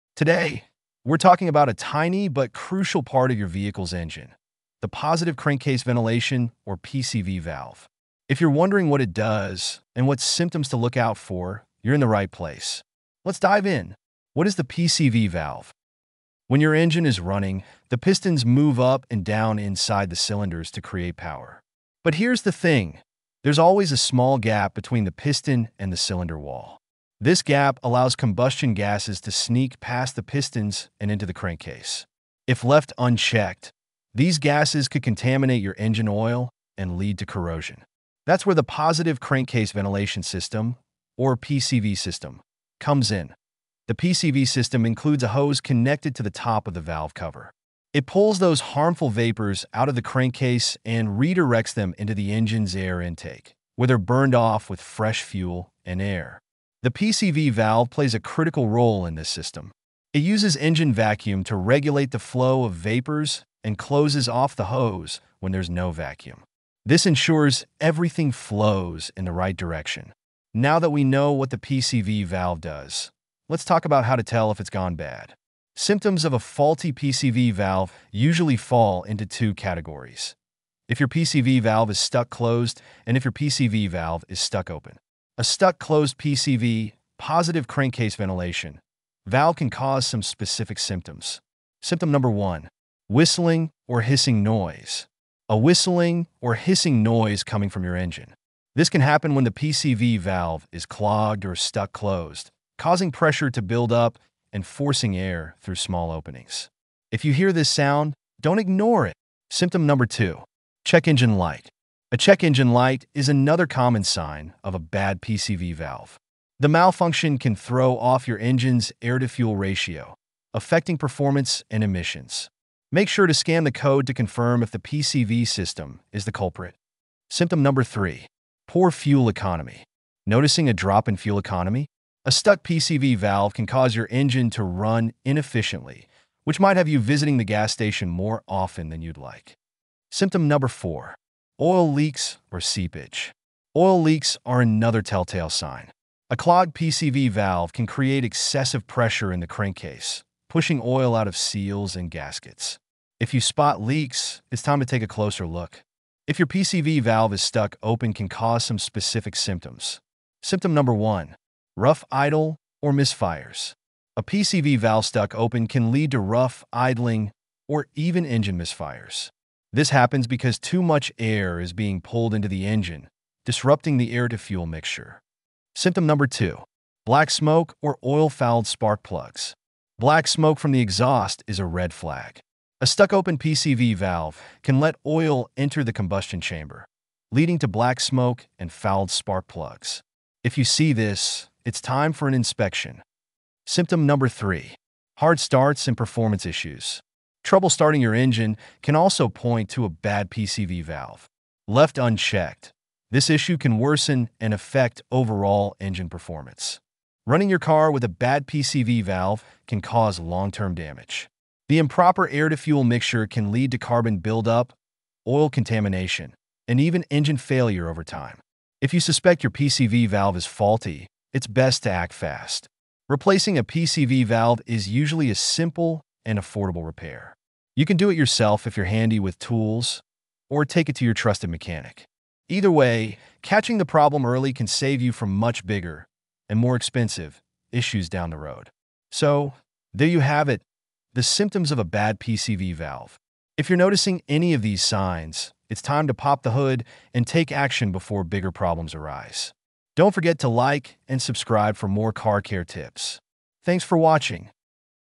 Top Symptoms of a Bad Positive Crankcase Ventilation (PCV) Valve You Shouldn't Ignore!
Is your car showing signs of trouble? A failing PCV valve might be the culprit! The Positive Crankcase Ventilation (PCV) valve plays a critical role in your engine’s performance and overall health. When it malfunctions, it can lead to a variety of issues that you shouldn't overlook.

In this video, we’ll cover:
✅ What the PCV valve does and why it’s so important.
✅ Key symptoms of a bad PCV valve, including:

Whistling or hissing sounds from the engine
Rough idle or frequent misfires
Oil leaks or buildup in the PCV system
Increased oil consumption
Difficulty starting the engine
Higher emissions and poor fuel efficiency
Learn how to spot these warning signs early, avoid costly repairs, and protect your engine from potential damage caused by a bad PCV valve. We’ll also discuss diagnostic tips and when it’s time to replace this small but essential component.